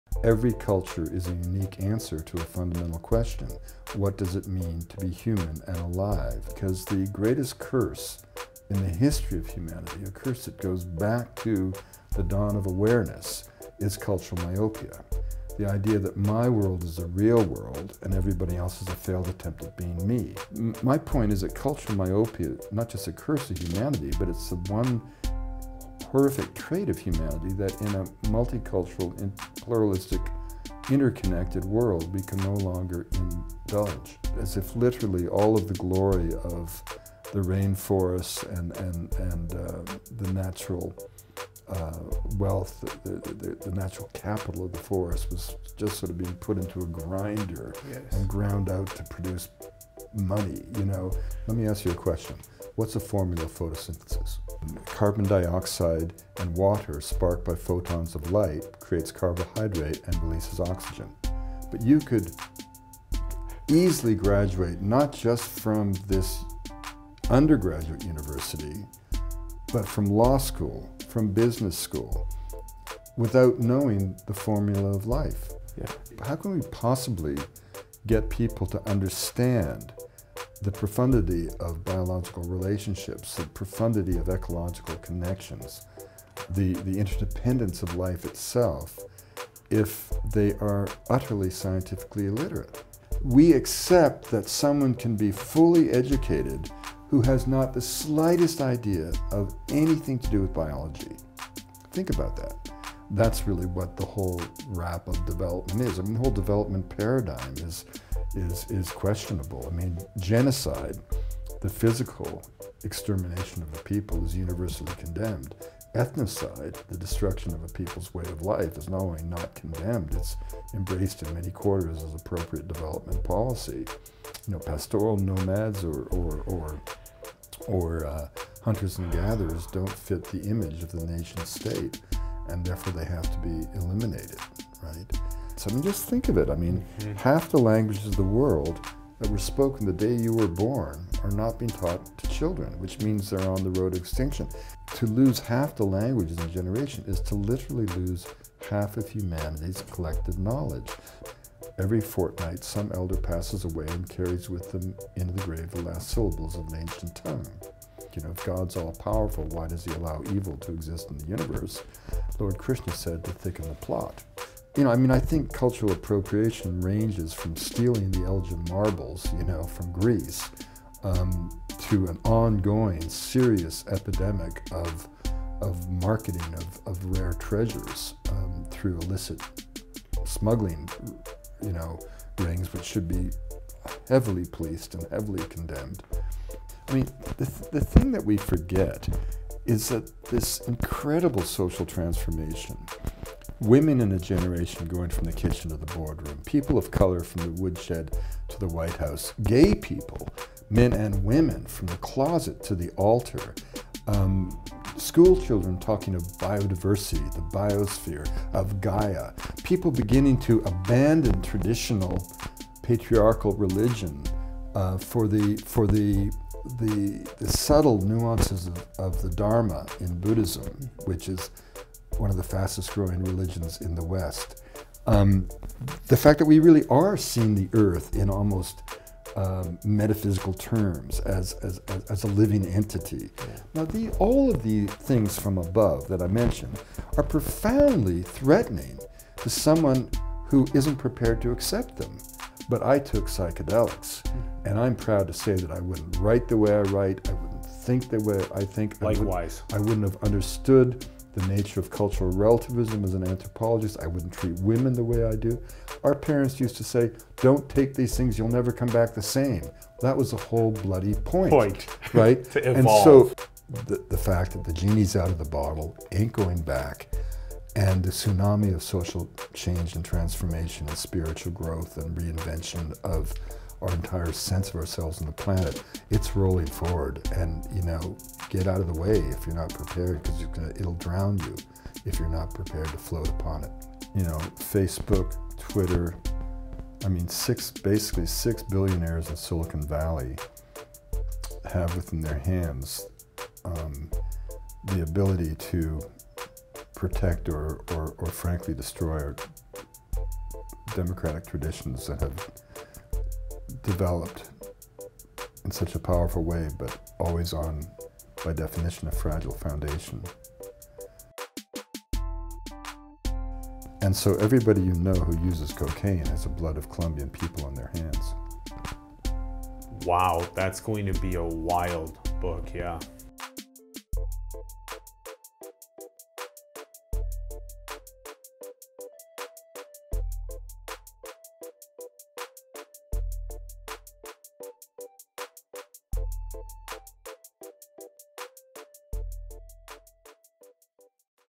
Cultural Myopia & Ethnographic Condensation | Dr. Wade Davis
Dr. Wade Davis is a World-Renowned Anthropologist and Ethnobotanist focused on Worldwide Indigenous Cultures & Psychoactive Plants. He’s a Multiple Time Award-Winning Author, Filmmaker, Photographer, former National Geographic Explorer in Residence, Professor of Anthropology, and BC Leadership Chair in Cultures & Ecosystems at Risk at the University of British Columbia. His work has taken him from the Amazon to the Arctic, from Africa to Polynesia. He is a member of the Order of Canada and recently became an Honorary Colombian Citizen.

1 Interview Smart People
2️ Synthesize First Principles
3️ Elevate Planetary Consciousness

👀 SOCIAL

🎧 AUDIO

💰SUPPORT

🇨🇳 CHINA

👕 MERCH

🎤 PUBLIC SPEAKING

❓ QUESTIONS 📹 HIRING